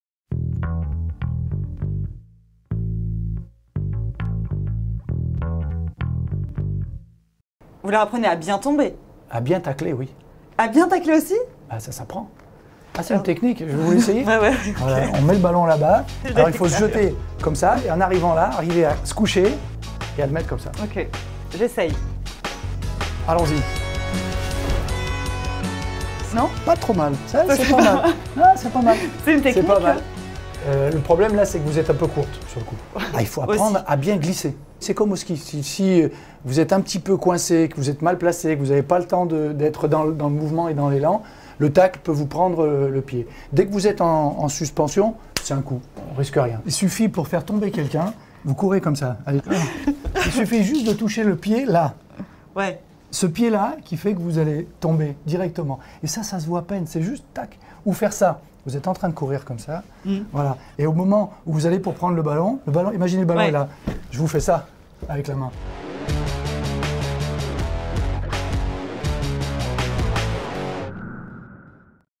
Exclusif, Raymond Domenech: Comment tacler ?
Raymond Domenech nous explique que tacler, cela s'apprend aussi...